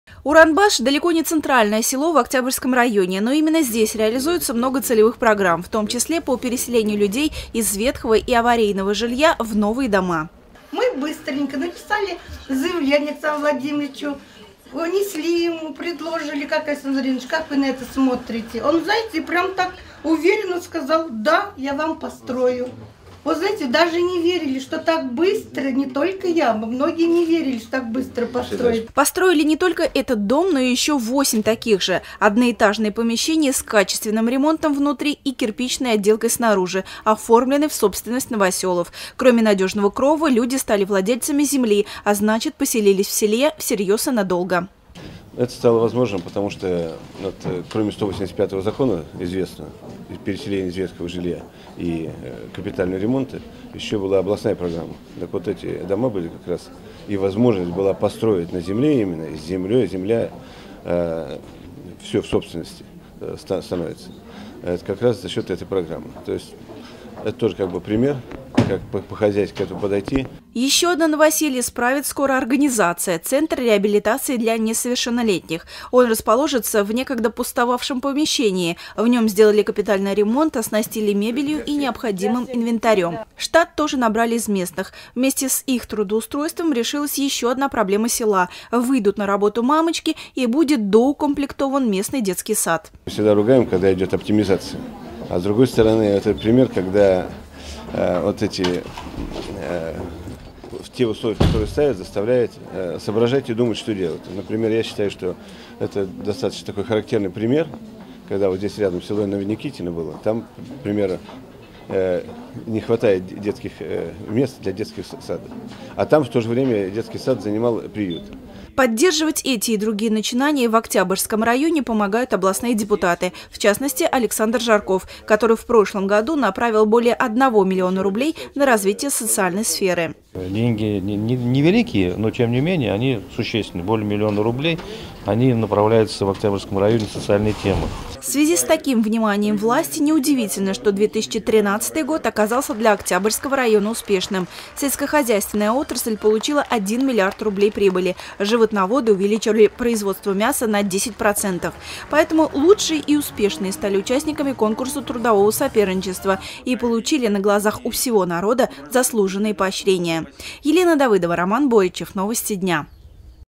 Кому - новоселье, а кому и премия
В Октябрьском районе с рабочим визитом побывал  спикер оренбургского парламента Сергей Грачев, сначала он встретился с новоселами села Уранбаш, а  затем побывал на подведении итогов конкурса среди трудовых предприятий района.